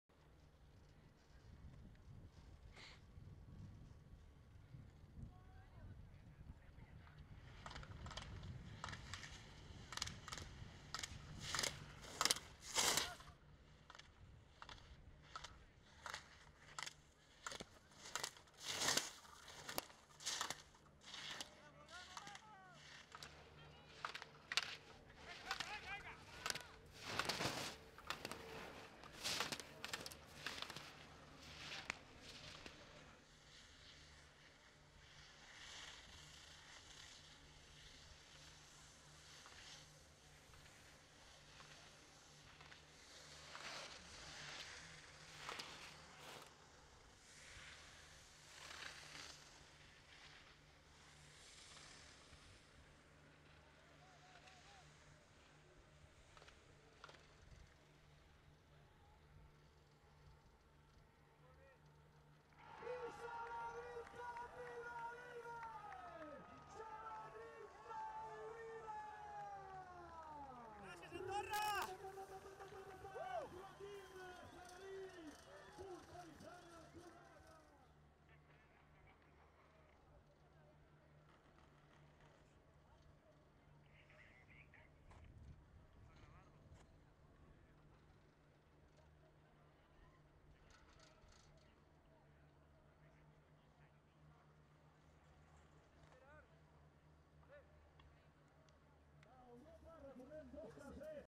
Joaquim Salarich segundo en el slalom de las Finales de la Copa de Europa de Grandvalira
El segundo clasificado de la carrera en Andorra ha sido el español Joaquim Salarich, con uno tiempo de 1:46.62, que firma su mejor resultado en la Copa de Europa de este año y cierra en 7ª posición general de la modalidad